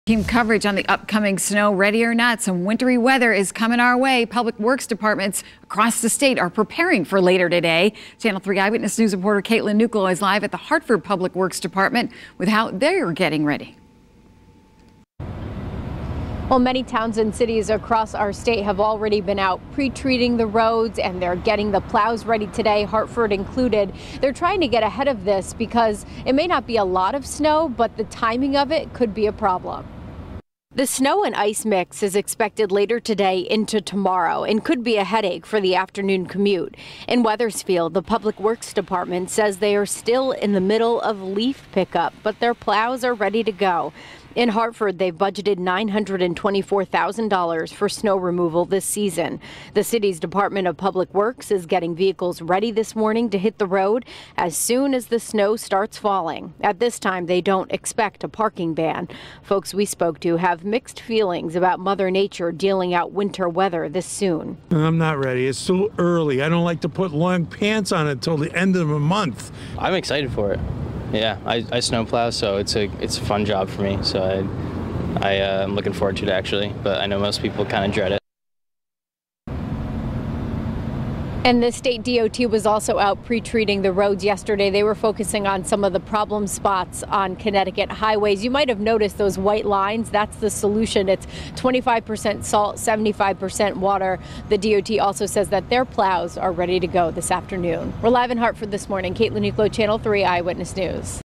VIDEO: DOT ready to go for snow
The state Department of Transportation said it has prepared for Thursday's snow.